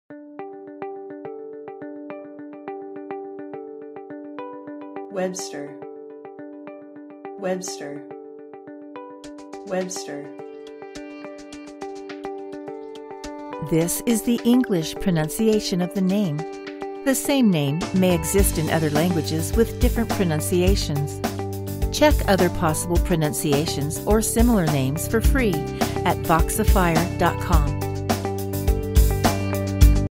How to Pronunce Webster in English - Voxifier.com
This is the English pronunciation, popularity, and phonetics recording of the surname Webster. The same name may exist in other languages with different pronunciations.